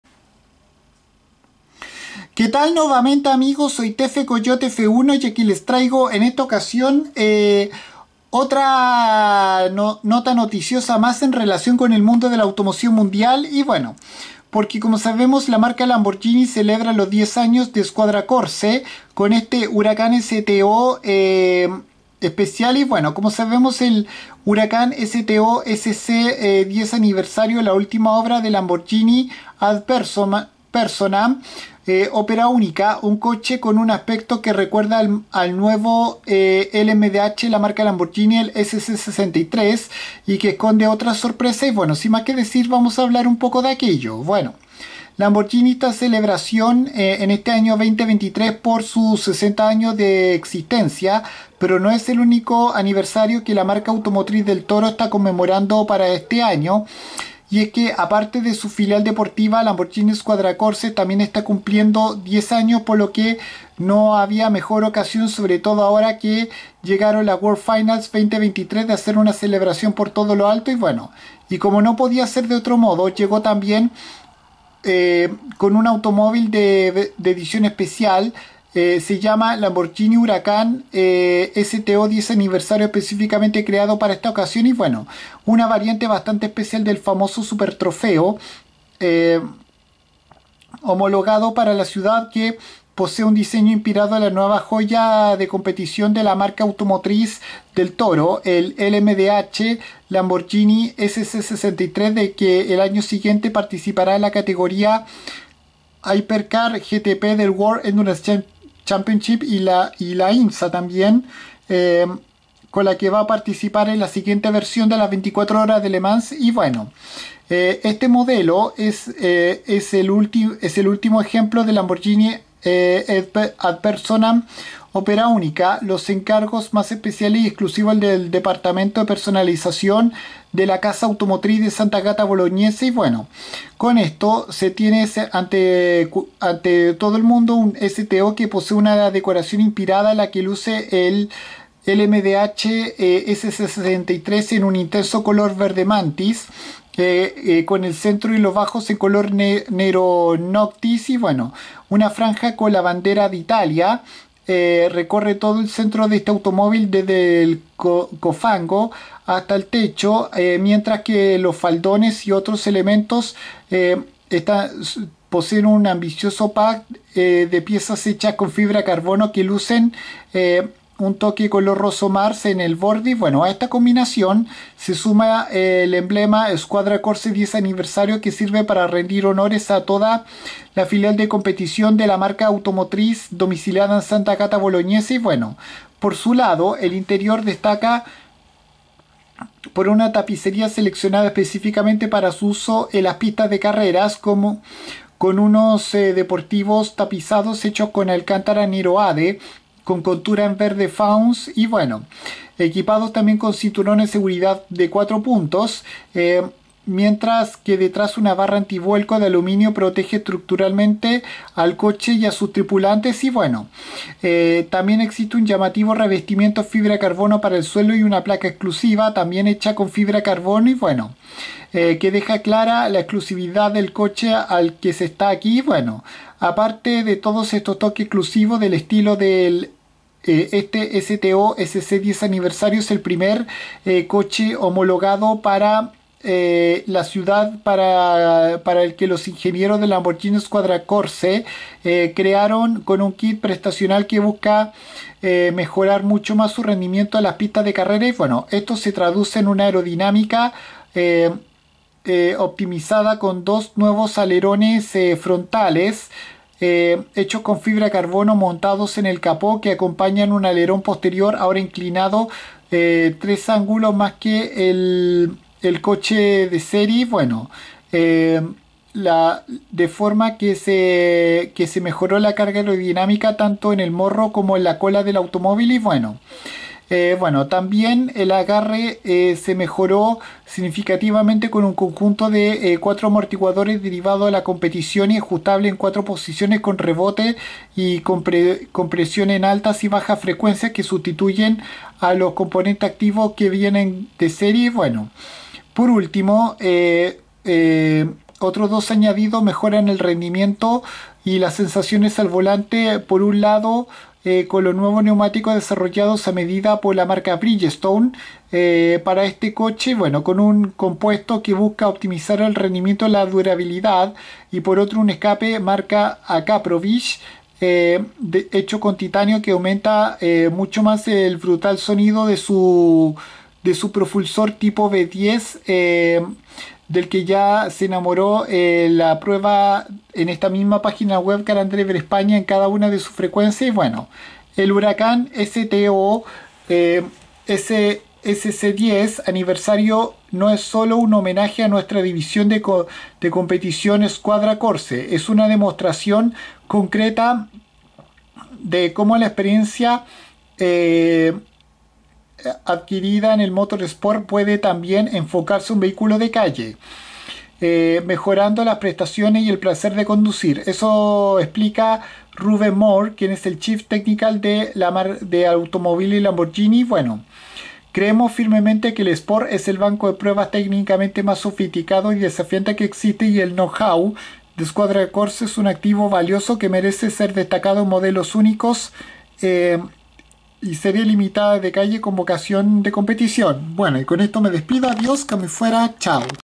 Lamborghini Huracán STO SC 10° Aniversario información
Con este especial modelo la marca automotriz del toro celebra los 10 años de existencia de su filial Squadra Corse y su decoración está inspirada en el SC63